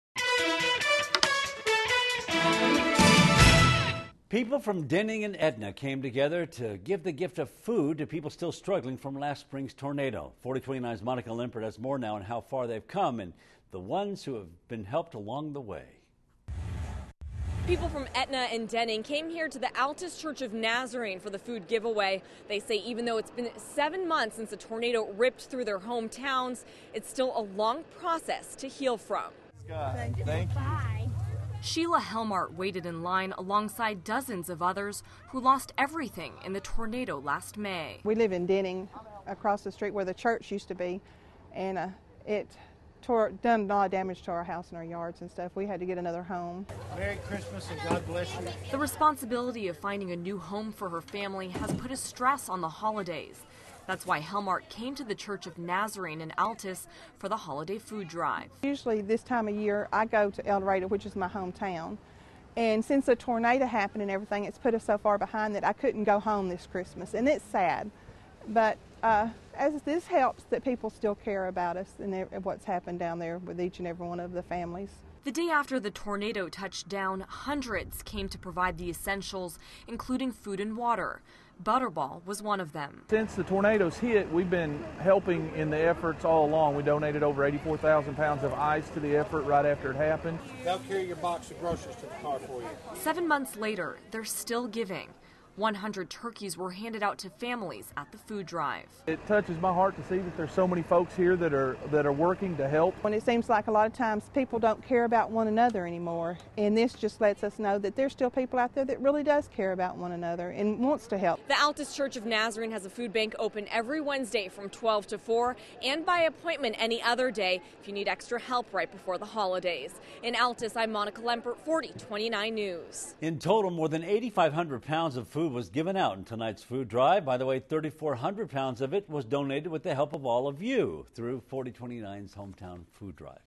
Tornado Victims Have Come A Long Way
People from Denning and Etna came together to give the gift of food to people still struggling from last Spring's tornado.